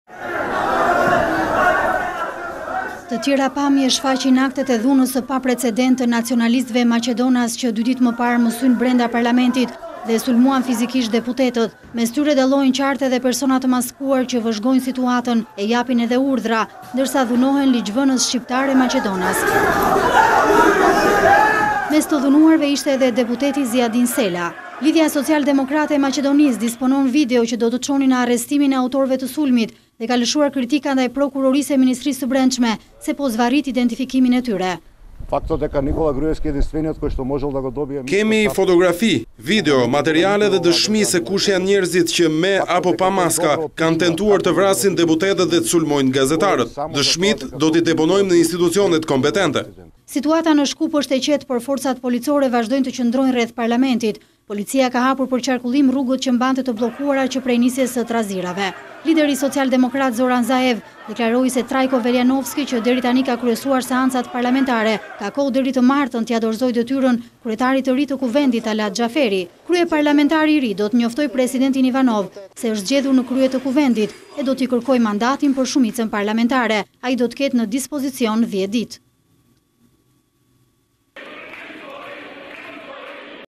Maskat që drejtonin rrahjen e deputetëve në Maqedoni - News, Lajme - Vizion Plus
Mes tyre dallohen qartë edhe persona të maskuar, që vëzhgojnë situatën e japin edhe urdhra, ndërsa dhunohen ligjvënës shqiptarë e maqedonas. Mes të dhunuarve ishte edhe deputeti Zijadin Sela. LSDM disponon video që do të çonin në arrestimin e autorëve të sulmit dhe ka lëshuar kritika ndaj Prokurorisë dhe Ministrisë së Brendshme se po zvarrit identifikimin e tyre. “Kemi fotografi, video, materiale dhe dëshmi se kush janë njerëzit, qe me apo pa maska kanë tentuar të vrasin deputetët e të sulmojnë gazetarët. Dëshmitë do t’i deponojmë në institucionet kompetente.” Situata në Shkup është e qetë, por forcat policore vazhdojnë të qëndrojnë rreth parlamentit. Policia ka hapur për qarkullim rrugët që mbante të bllokuara që prej nisjes së trazirave. Lideri socialdemokrat Zoran Zaev deklaroi se Trajko Veljanovski që deri tani ka kryesuar seancat parlamentare, ka kohë deri të martën t’ia dorëzojë detyrën kryetarit të ri të kuvendit, Talat Xhaferi. Kryeparlamentari i ri do të njoftojë presidentin Ivanov se është zgjedhur në krye të kuvendit dhe do t’i kërkojë mandatin për shumicën parlamentare. Ai do ketë ne dispozicion 10 ditë.